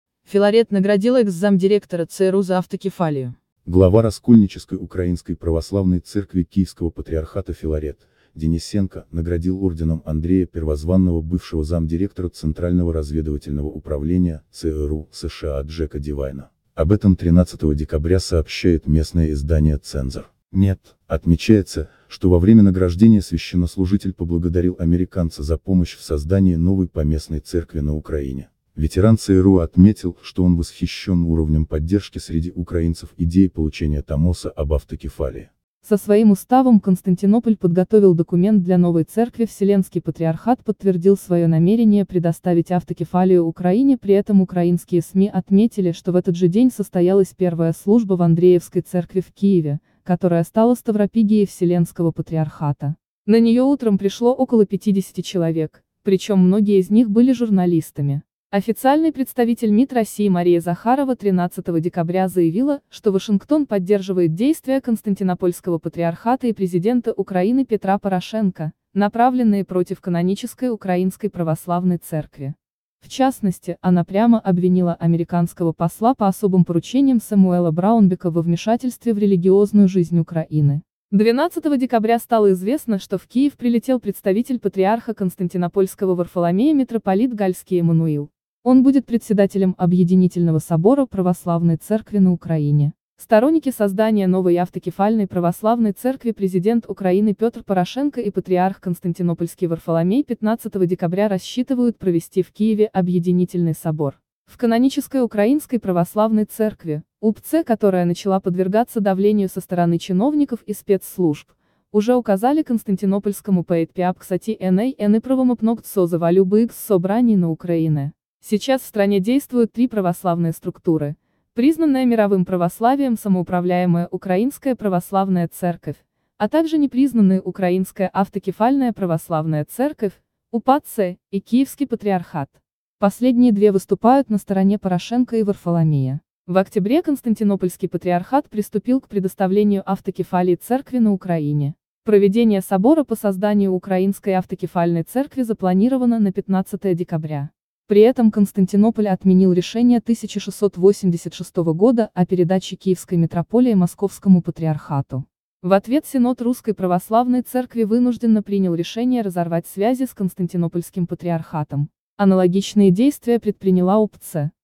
Филарет наградил экс-замдиректора ЦРУ за автокефалию
Филарет наградил экс-замдиректора ЦРУ за автокефалию Глава раскольнической украинской православной церкви Киевского патриархата Филарет Денисенко наградил орденом Андрея Первозванного бывшего замдиректора Центрального разведывательного управления ЦРУ США Джека Девайна . Об этом 13 декабря сообщает местное издание Цензор .нет . Отмечается, что во время награждения священнослужитель поблагодарил американца за помощь в создании новой поместной церкви на Украине . Ветеран ЦРУ отметил, что он восхищен уро